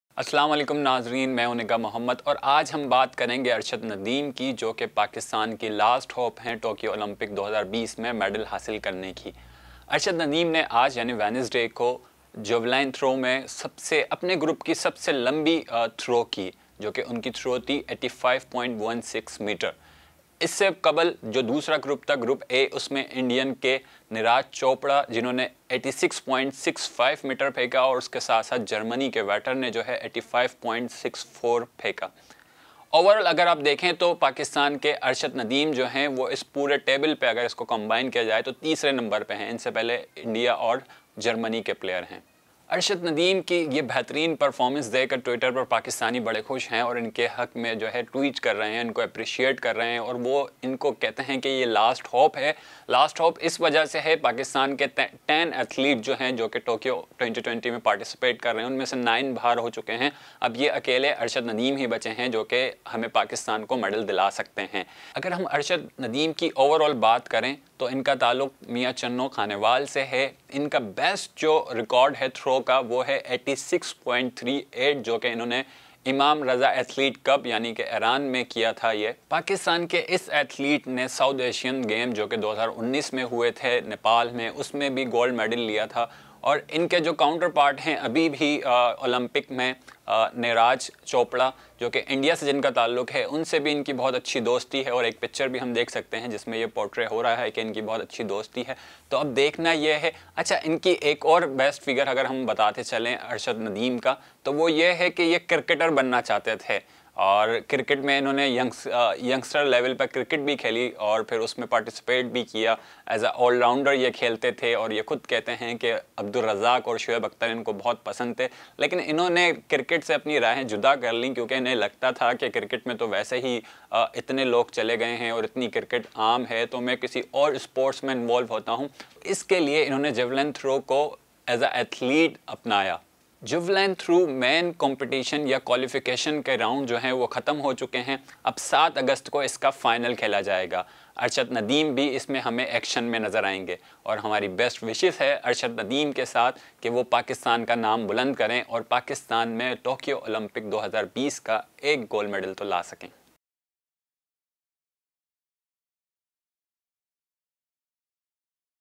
Arshad Nadeem keeps Pakistan's medal hope alive at Tokyo Olympics, reaches javelin throw final
Pakistani athlete Arshad Nadeem has kept the nation's hopes of winning a medal alive after he qualified for the finals of the javelin throw at Tokyo Olympics.